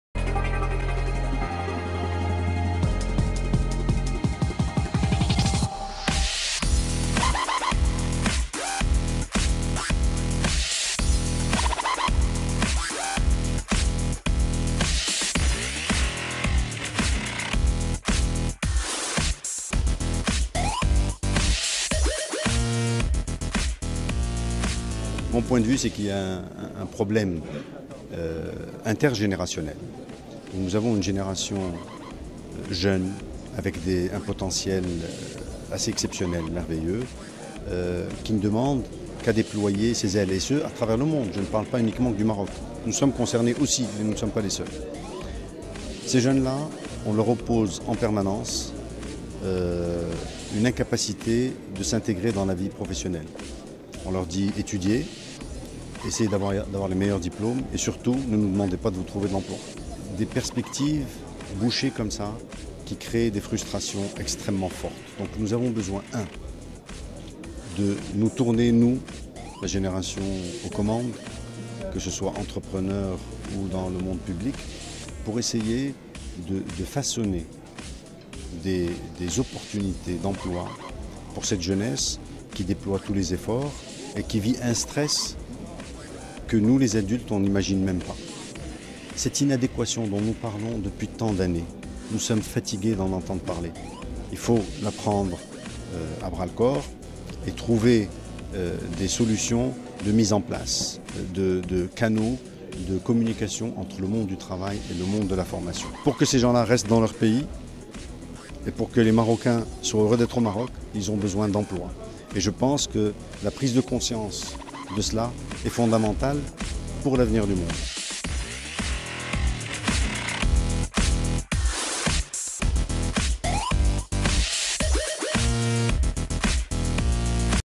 Moulay Hafid Elalamy au Forum de Paris Casablanca Round 2015
La jeunesse vit un stress que les adultes n'imaginent pas
Interview: Maha Basraoui et Chaib Hammadi